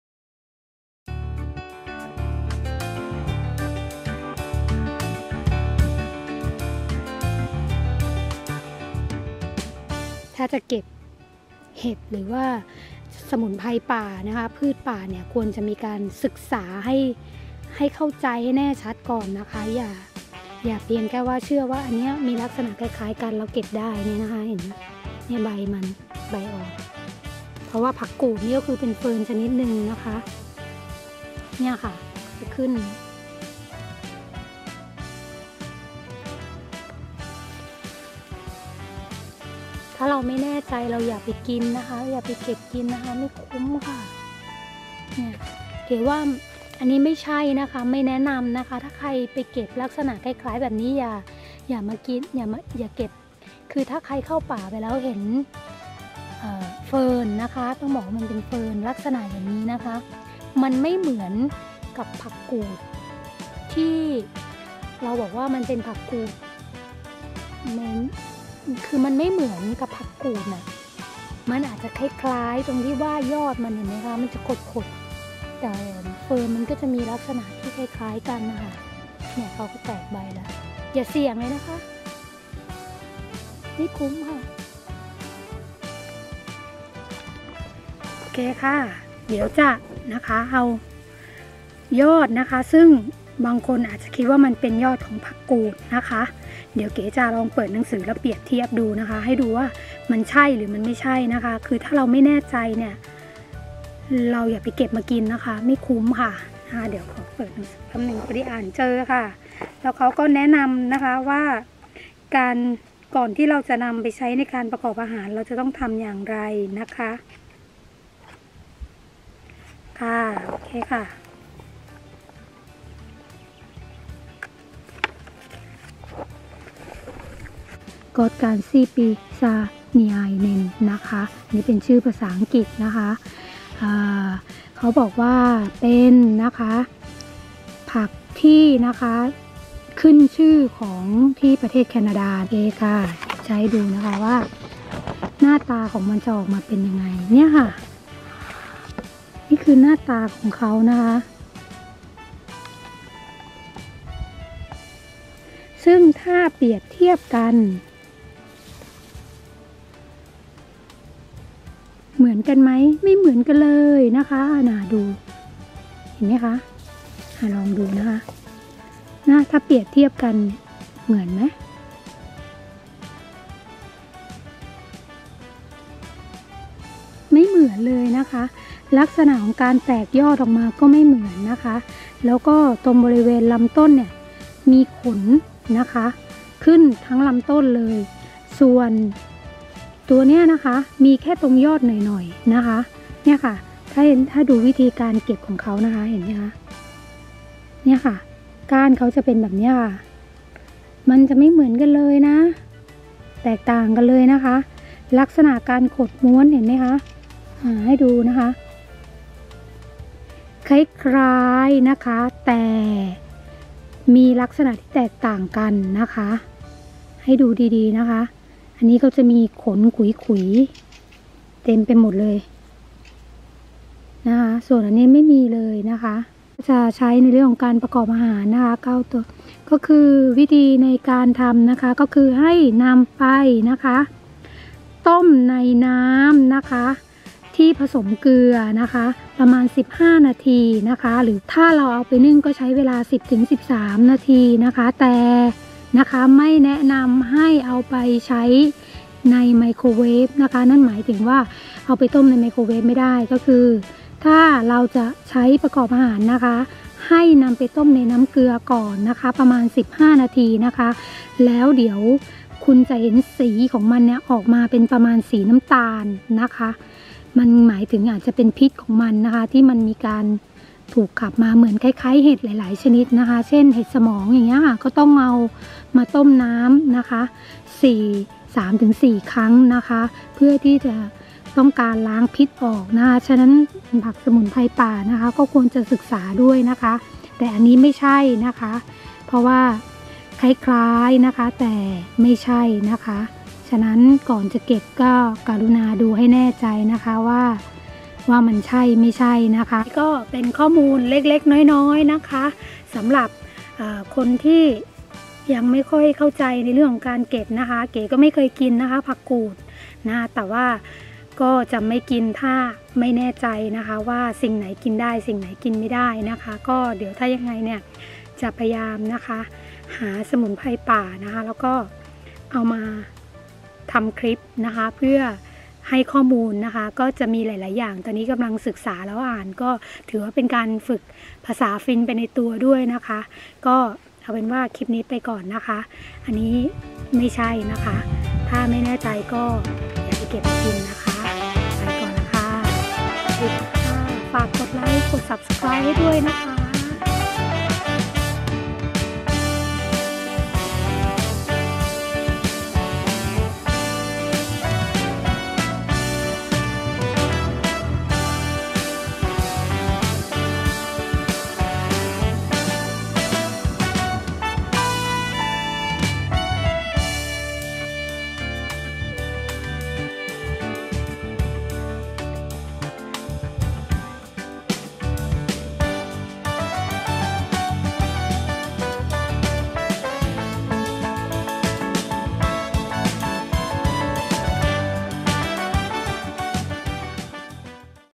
เฟิร์นชนิดนี้ ไม่ใช่ ผักกูด อันตราย☠️🤮!!ชีวิตต่างแดน
ผักกูด Ostrich Fern (Kotkansiipi)เป็นเฟิร์นชนิดหนึ่งที่ขึ้นตามธรรมชาติ มีให้เห็นมากมายในป่าเฟิร์นซึ่งจะเจริญเติบโตได้ดีใกล้แหล่งน้ำ  การเก็บผักกูดมาบริโภคควรมีการศึกษาและหาข้อมูลก่อน เพราะเฟิร์นจะมีลักษณะคล้ายกัน
ฝากกดไลค์ติดตามวีดีโอช่อง YouTube เพื่อเป็นกำลังใจให้เก๋ได้ทำคลิปวีดีโอต่อไปเรื่อยๆนะคะ ขอบคุณมากค่ะ